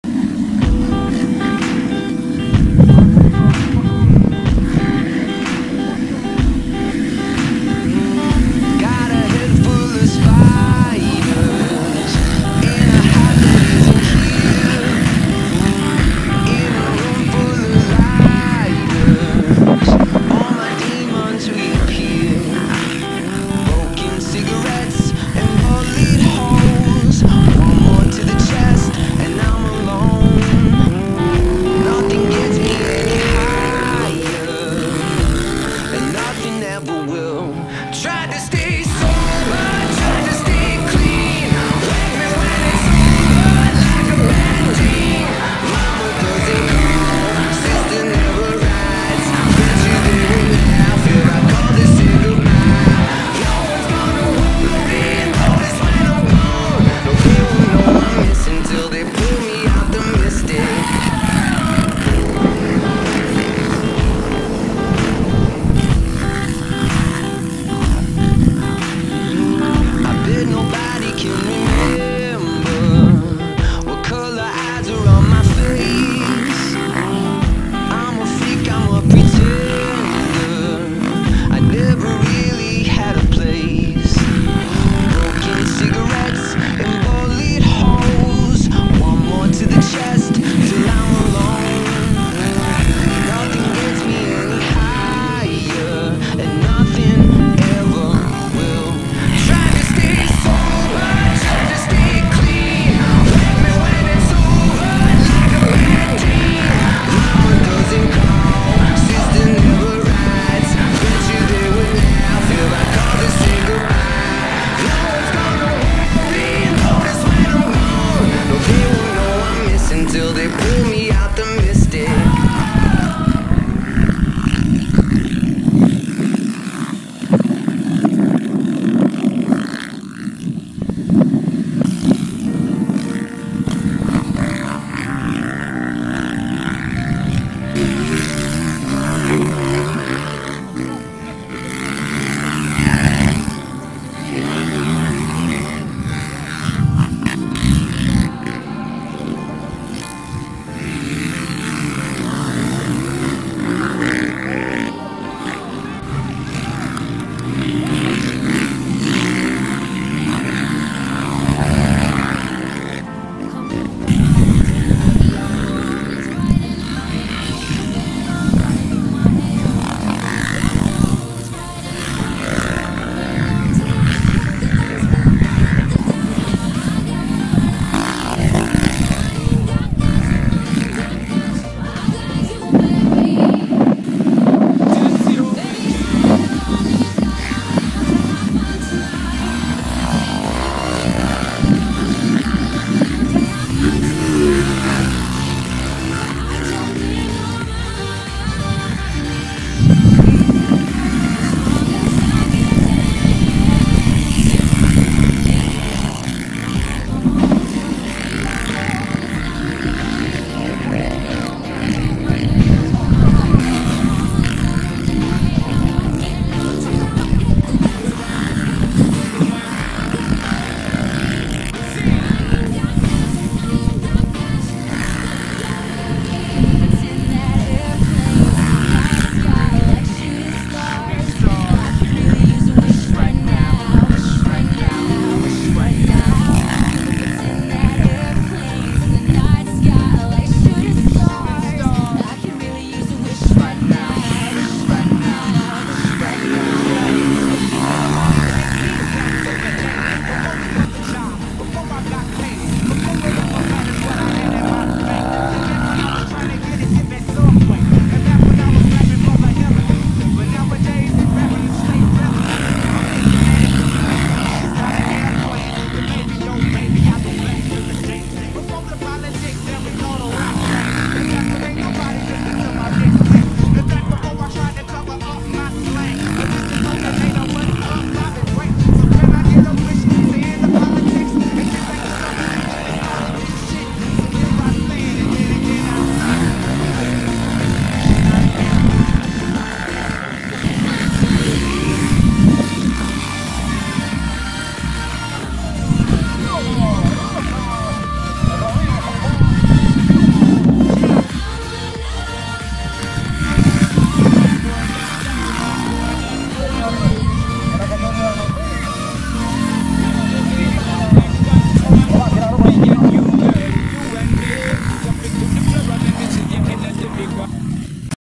8va. Fecha del Campeonato Departamental de Motociclismo 2022
COPA: “ADMSC".
Modalidad Enduro
Categoria:Cg-Junior
1)Diago Rojas
2)Jose Carlos Liquitaya
3)Jose Andres Conde
Damas
1)Carla Tapia
2)Maria Rene Padilla
3)Vanesa Flores